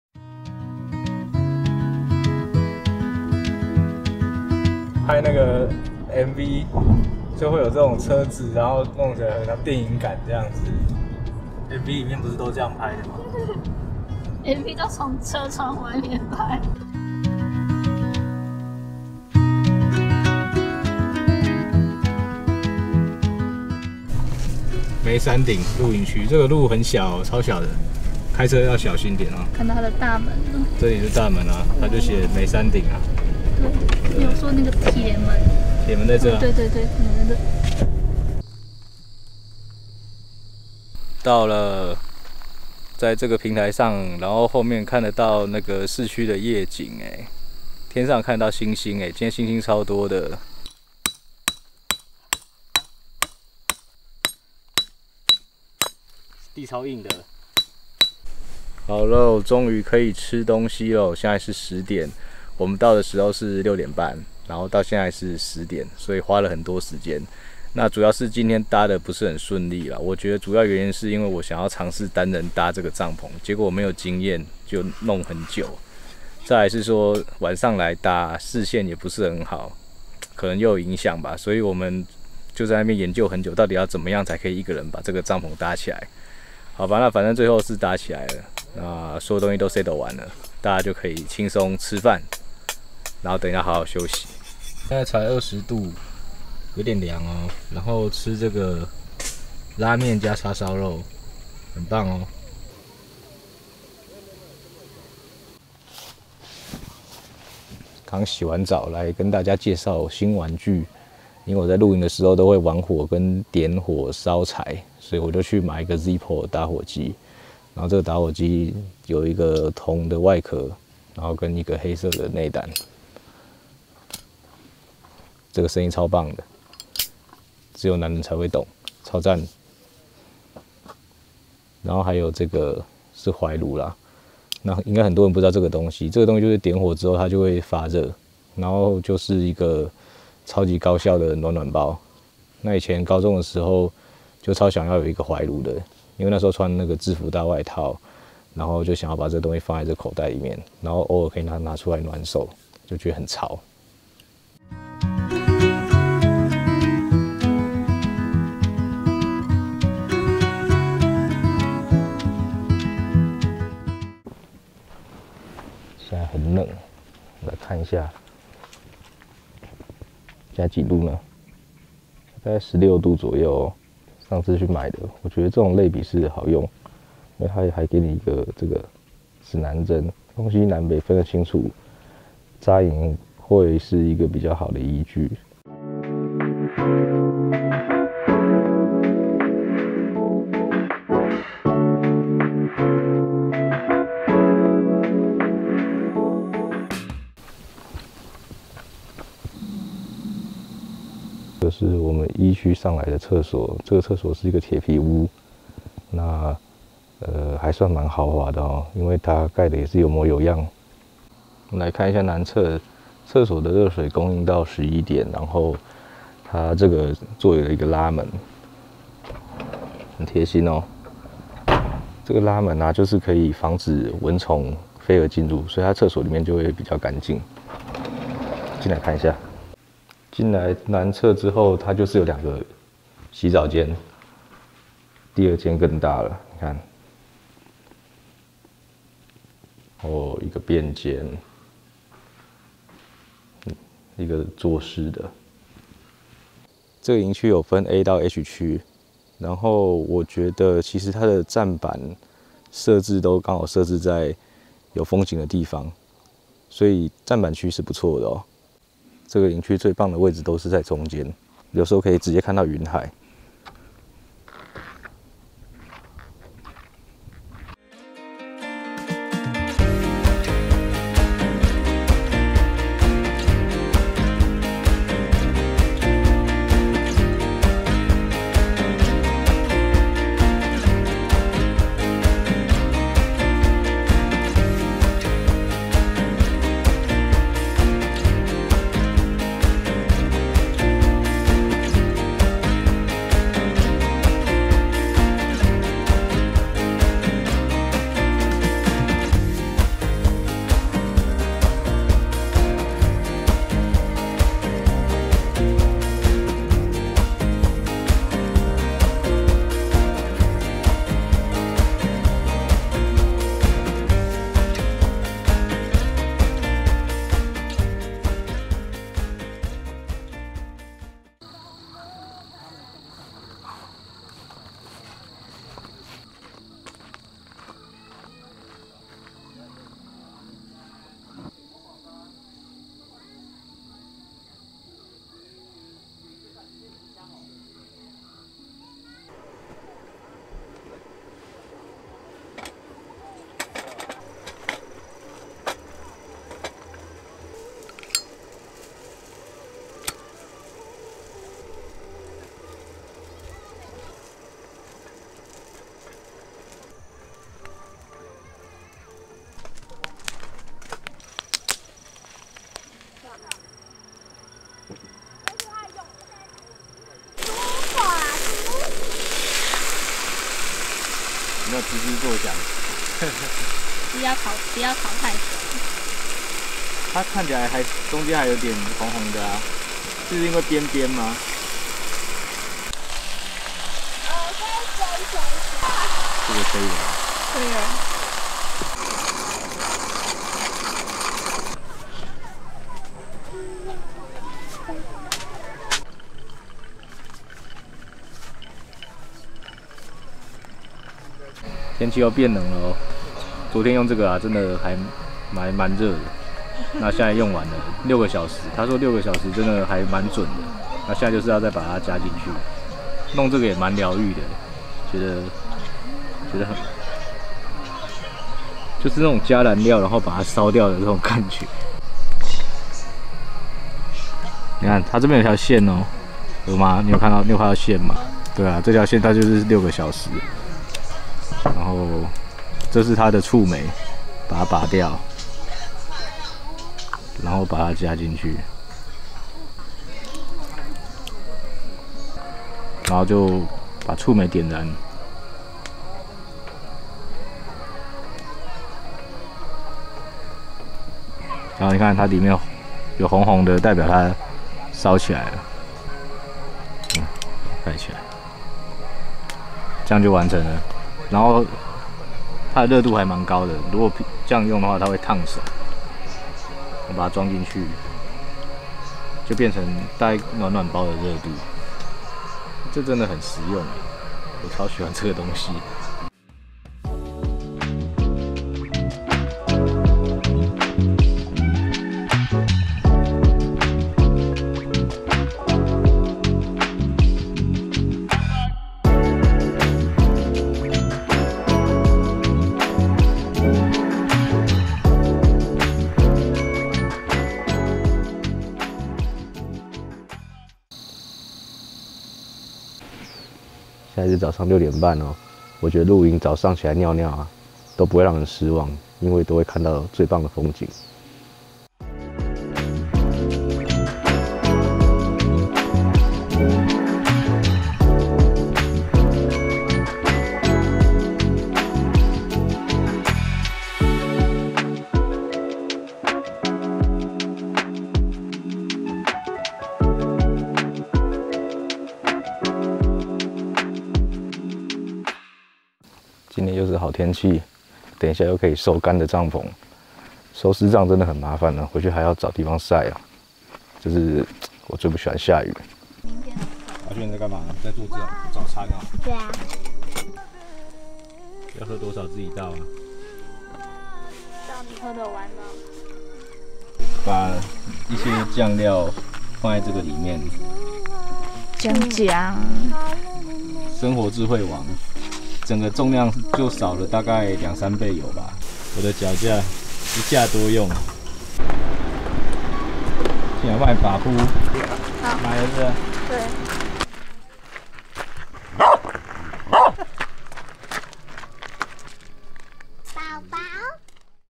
蘭斯露營(CC字幕) - 梅山頂露營區｜新竹縣五峰鄉｜Zippo打火機跟懷爐初嘗試
這次一樣晚上去，想試著單人搭帳，但不得要領搞了很久。
但我下次大概知道怎麼做了，下次再挑戰。
秋天到了，可以開始玩保暖了喔，入手了想要好久的Zippo打火機跟懷爐，冷冷的天握著懷爐實在有夠舒適的啦。